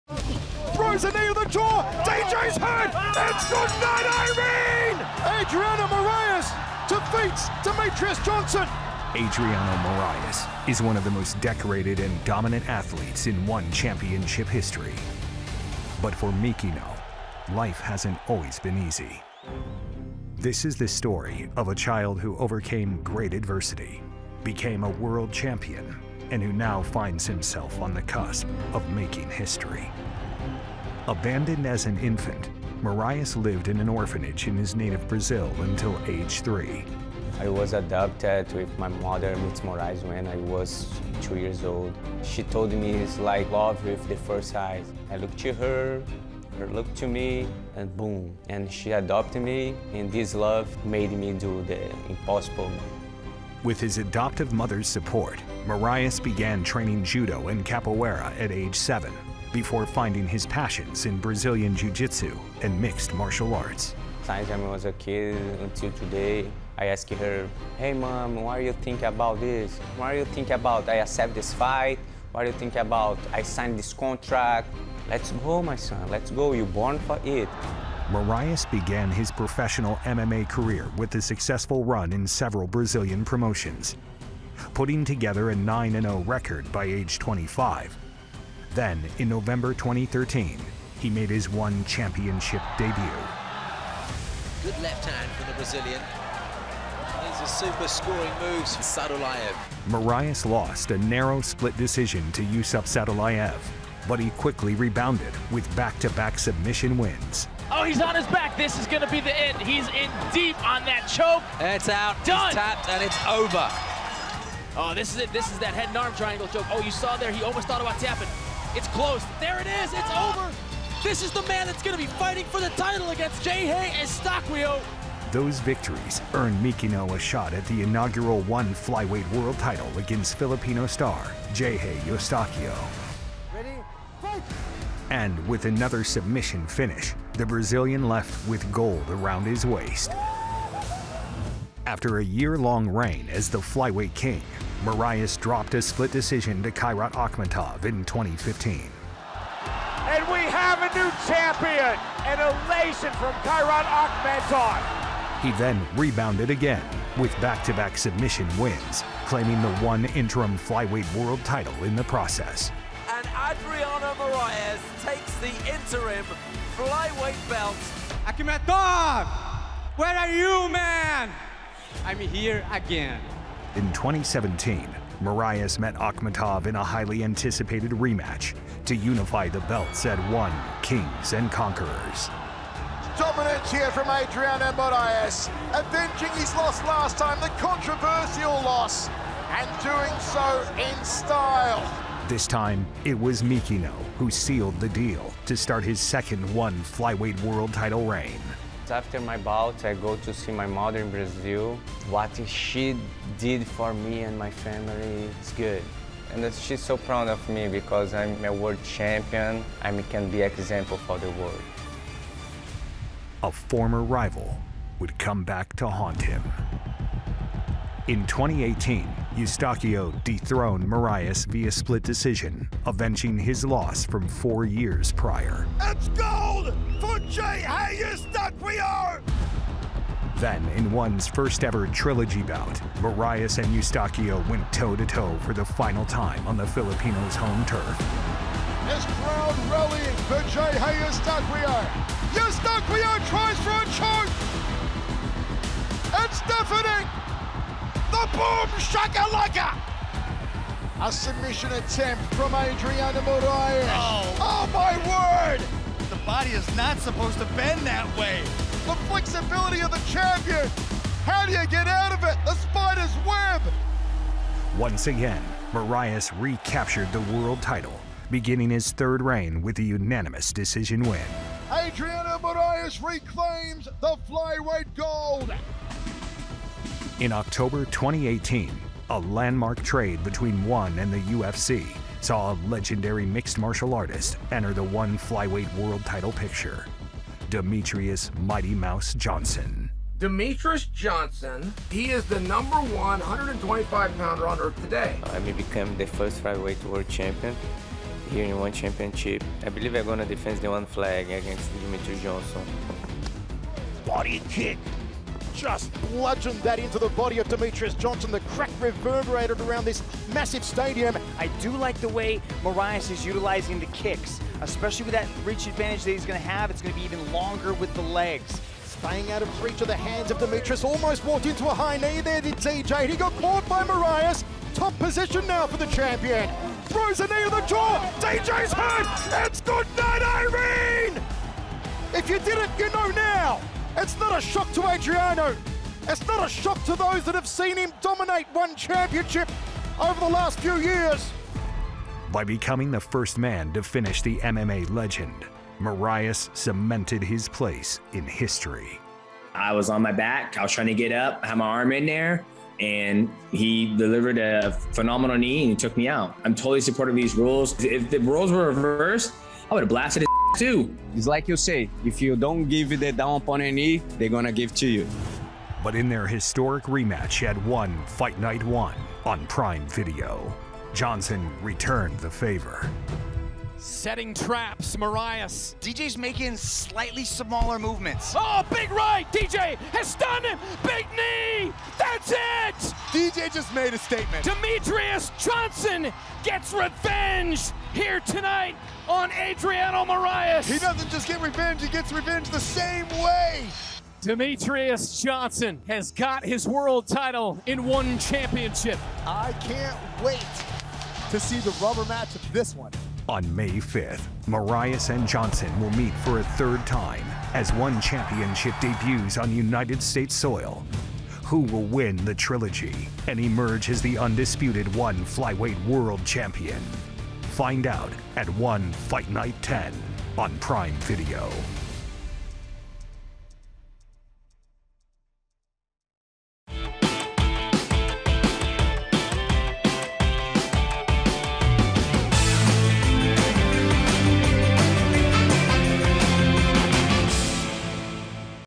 Adriano Moraes' Journey to Success | ONE Championship | Prime Video
Look back on Brazilian superstar Adriano Moraes' incredible tenure in ONE Championship ahead of his titanic ONE Flyweight World Title trilogy clash with reigning king Demetrious Johnson at ONE Fight Night 10 on Prime Video on May 5!

ONEFightNight10 | May 5 at 8PM ET/5PM PT on Prime